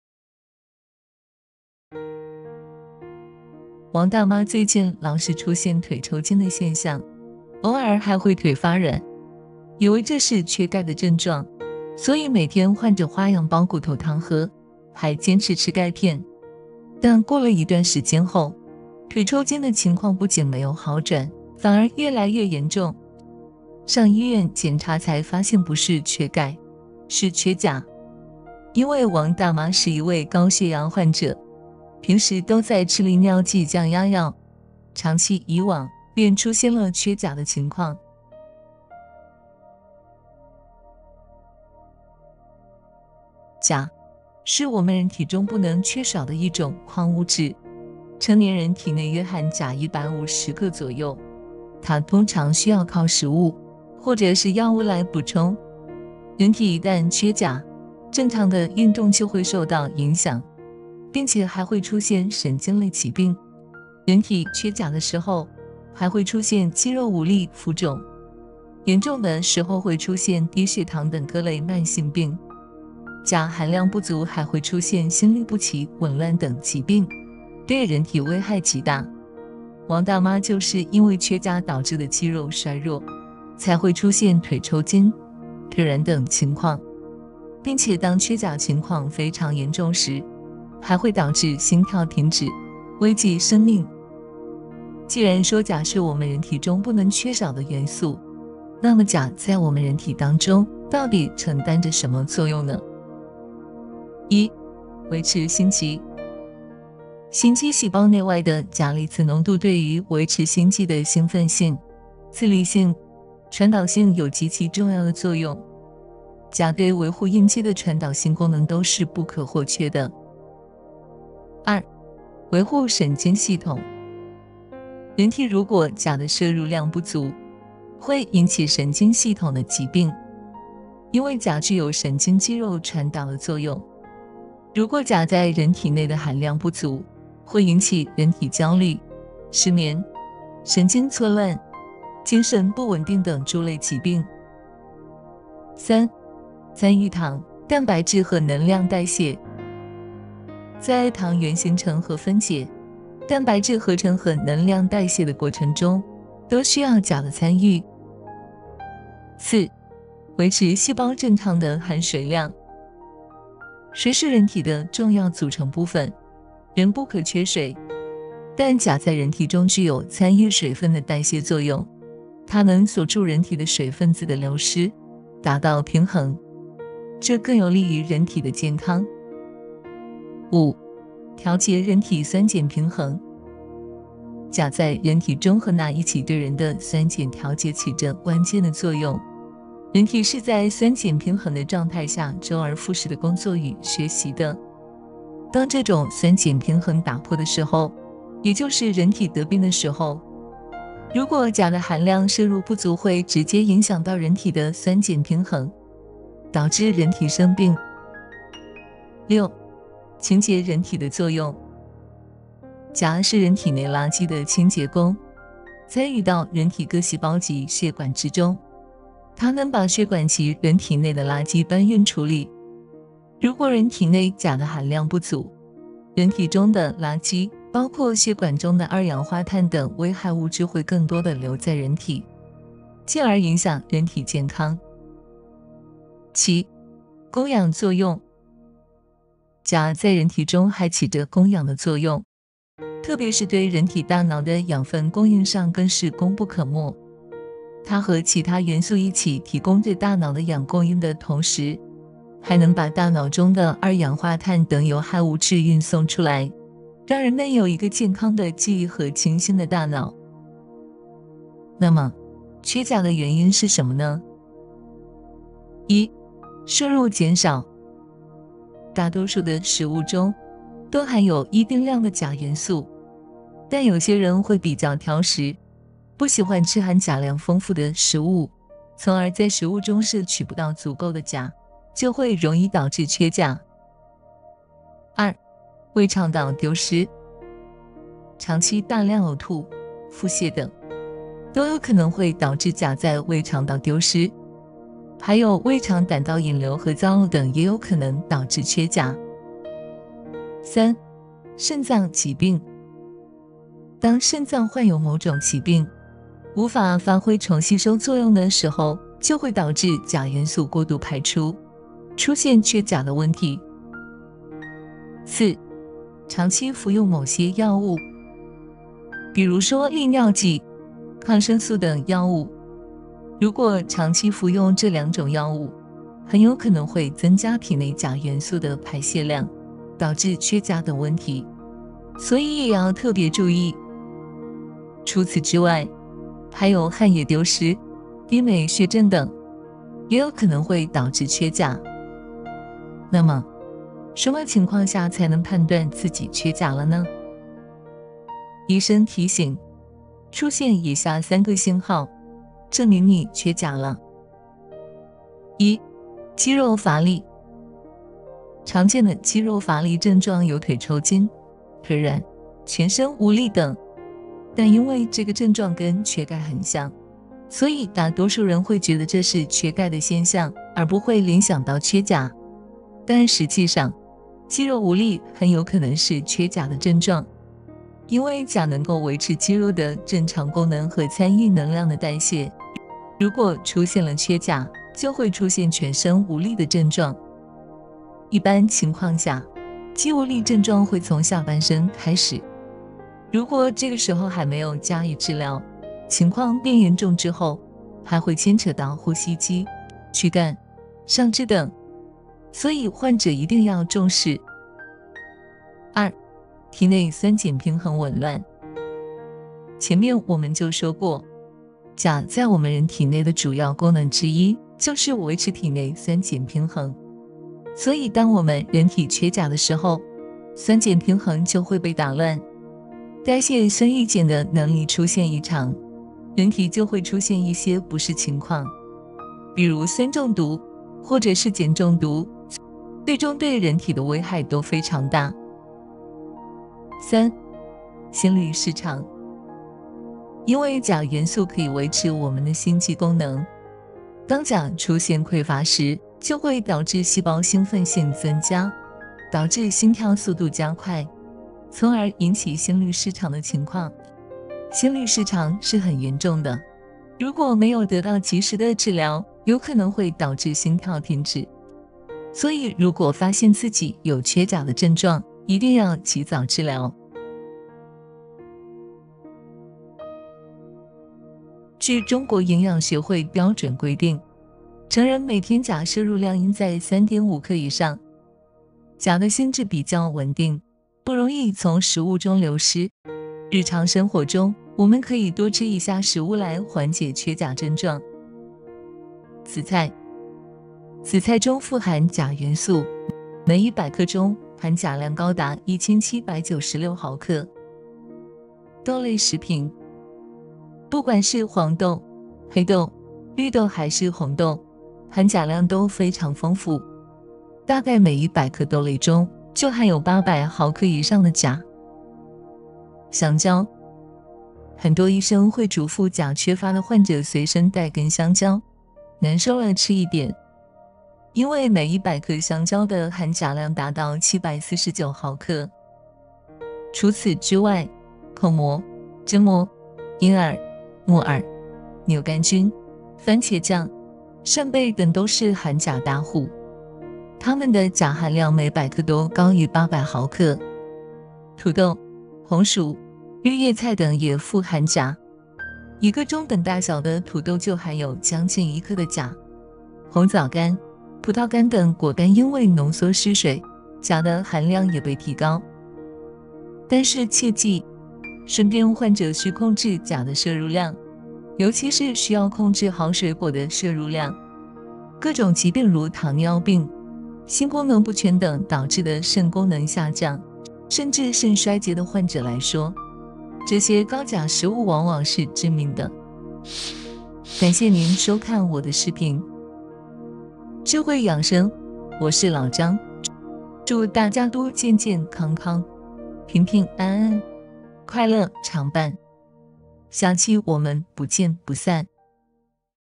腿一抽筋就补钙是大错特错！缺钾比缺钙更危险，这类人补错会致命！缺钾的原因/医生提醒出现3个信号，证明你缺钾了/日常饮食怎样补钾好
医生提醒，缺钾比缺钙更危险

钾，是我们人体中不能缺少的一种矿物质，成年人体内约含钾150克左右，它通常需要靠食物，或者是药物来补充。人体一旦缺钾，正常的运动就会受到影响，并且还会出现神经类疾病，人体缺钾的时候，还会出现肌肉无力、浮肿、严重的时候会出现低血糖等各类慢性病，钾含量不足还会出现心律不齐、紊乱等疾病，对人体危害极大。王大妈就是因为缺钾导致的肌肉衰弱，才会出现腿抽筋，腿软等情况，并且当缺钾情况非常严重时，还会导致心跳停止，危及生命。
Doctors warn that potassium deficiency is more dangerous than calcium deficiency

Potassium is a mineral we can not lack in the human body, the adult body contains about 150 grams of potassium, it usually needs to rely on food, or drugs to supplement. Once the human body is short of potassium, normal exercise will be affected, and there will be neurological diseases, when the human body is short of potassium, there will be muscle weakness, edema, severe hypoglycemia and other chronic diseases, insufficient potassium content will also appear arrhythmia, disorders and other diseases, great harm to the human body. It is because of weak muscles caused by potassium deficiency that Wang's legs will suffer from cramps, soft legs and other conditions, and when the potassium deficiency is very serious, it can lead to heart arrest and life-threatening.